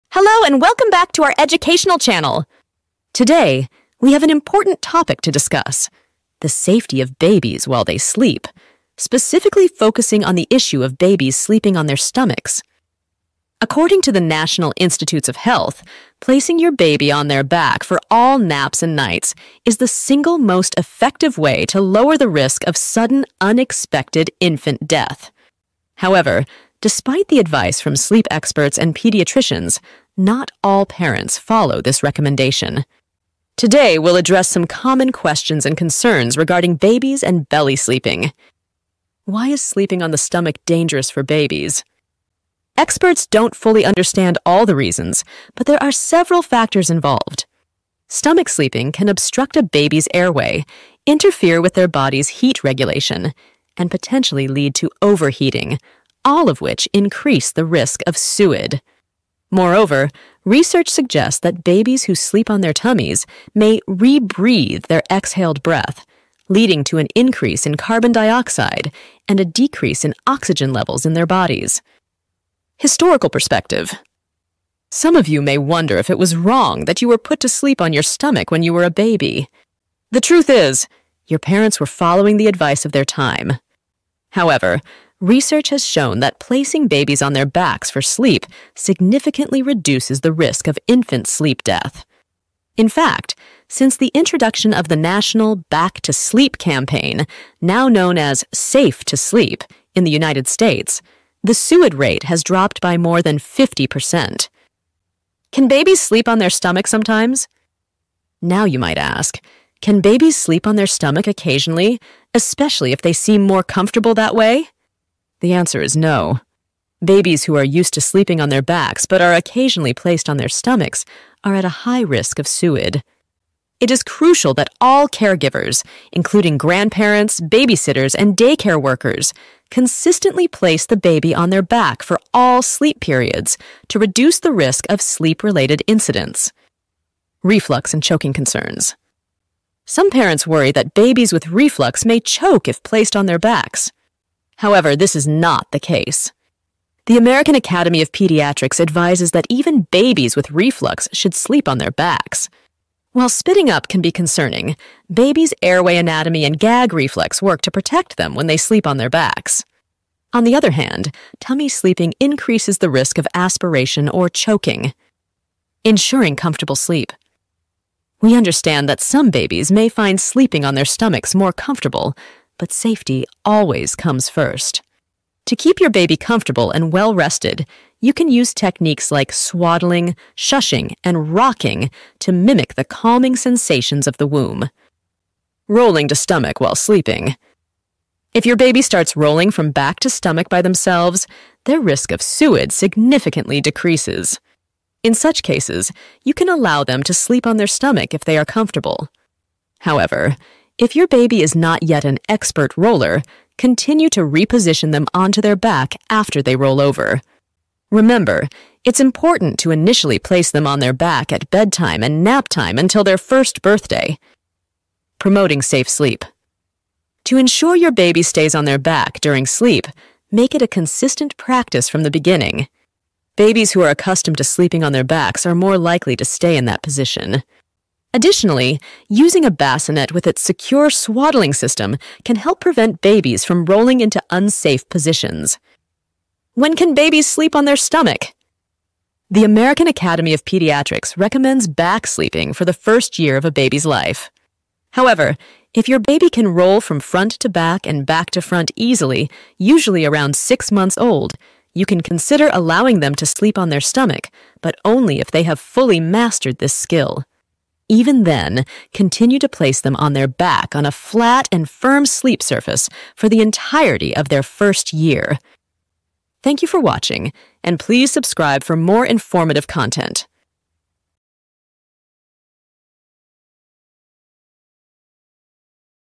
Why Is Sleeping On The Stomach Dangerous For Babies?
Learn why it's crucial to always place your baby on their back for sleep to reduce the risk of Sudden Unexpected Infant Death (SUID). Discover the dangers of stomach sleeping for infants and get expert advice on safe sleep practices.

Time stamps.

0:00 intro
0:39 why is sleeping on the stomach dangerous for babies?
1:13 historical perspective
1:45 can babies sleep on their stomach sometimes?
2:18 reflux and choking concerns
2:45 ensuring comfortable sleep
3:06 rolling to stomach while sleeping
3:35 promoting safe sleep
3:56 when can babies sleep on their stomach?